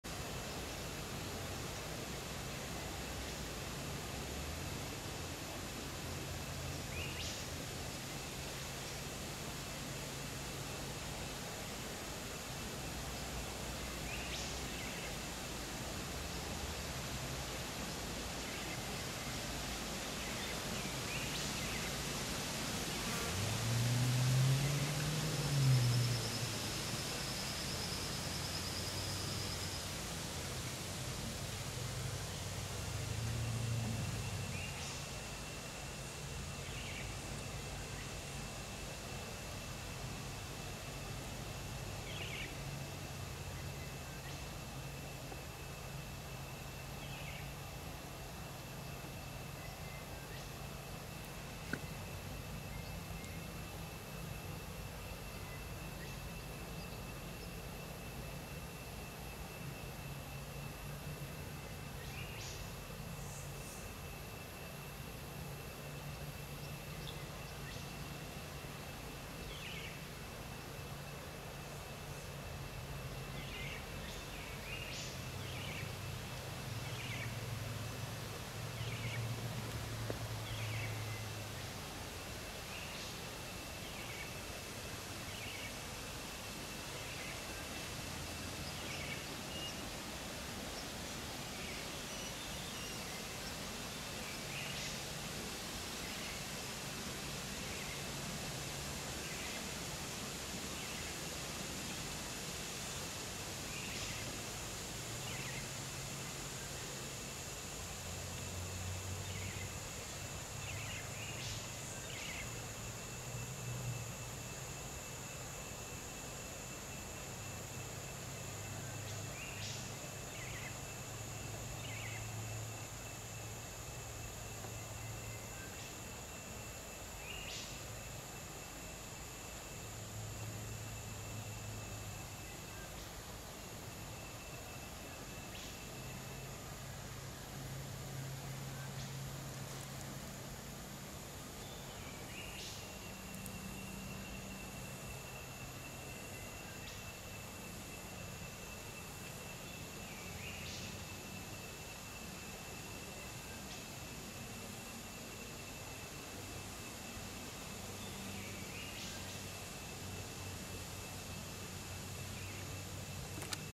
Australian Ants - A colony of Camponotus species ants from the Sunshine Coast.
A colony of Camponotus that I came across in Ewen Maddock dam in Landsborough, Qld. I don't know what species of Camponotus this is, I have previously seen individuals before but never found a colony. If someone knows what they are could they let me know.